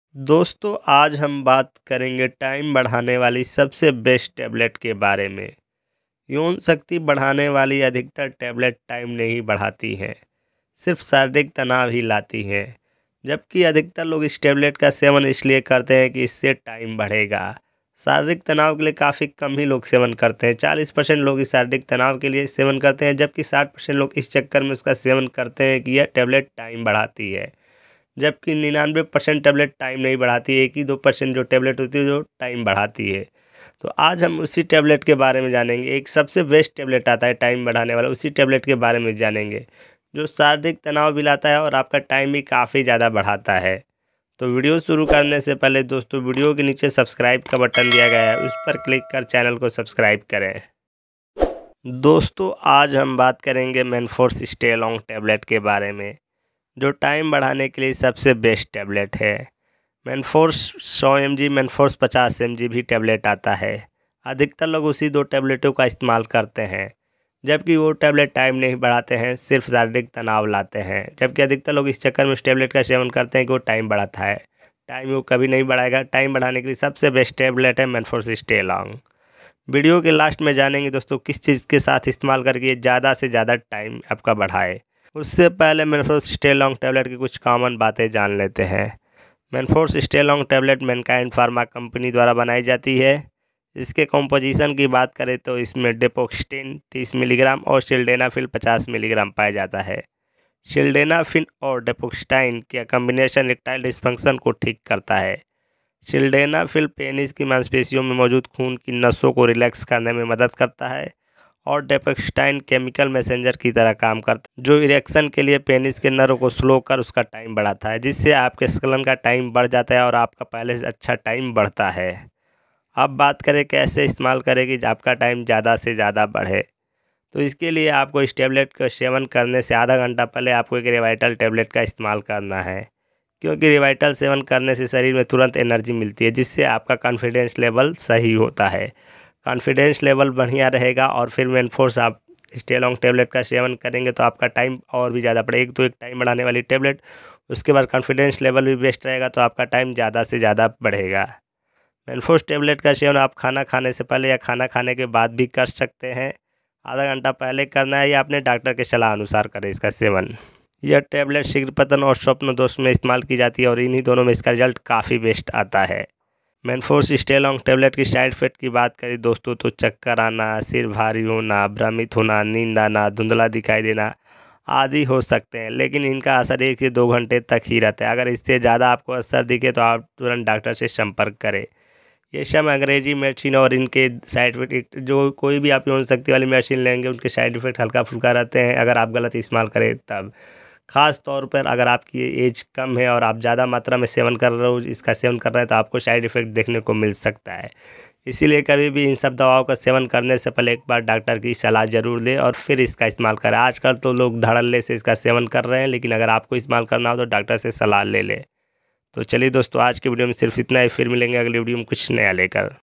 रात को बेड पर जाने से पहले 1 टेबलेट खा लो टाइम 1 घंटे तक हो सकता है | Manforce Staylong Tablet
Thanks & Regards
Sonu thakur